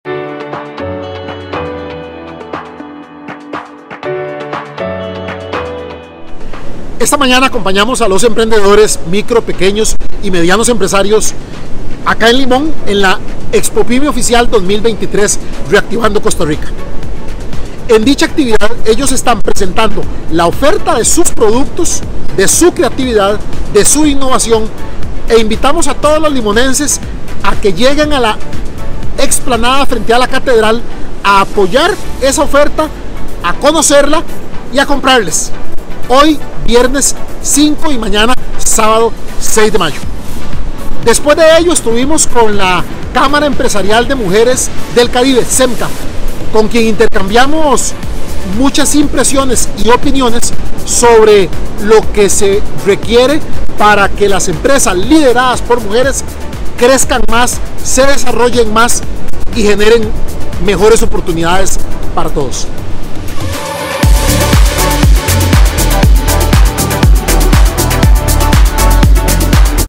Balance: Expopyme en Limón del ministro del MEIC, Francisco Gamboa
La inauguración de la expopymeoficial2023 en Limón y una reunión con las integrantes de la Cámara Empresarial de Mujeres del Caribe (CEMCA), con quienes intercambiamos opiniones e impresiones sobre lo que se requiere para que empresas lideradas por mujeres crezcan y se desarrollen más y generen mayores oportunidades para todos, fue lo que realizamos distante la visita que hizo el ministro del MEIC, Francisco Gamboa Soto a esa provincia caribeña.